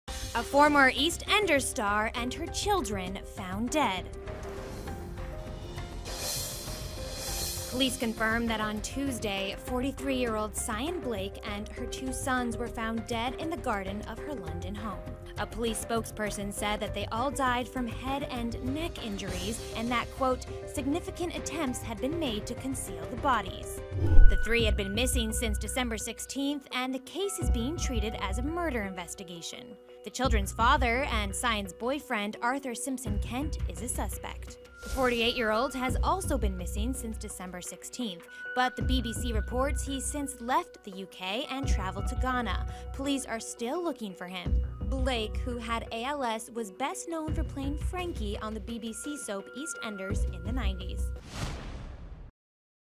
Former 'EastEnders' Actress Sian Blake and Her Sons Found Dead
Police confirm that Sian and her sons were found dead at the late actress' home.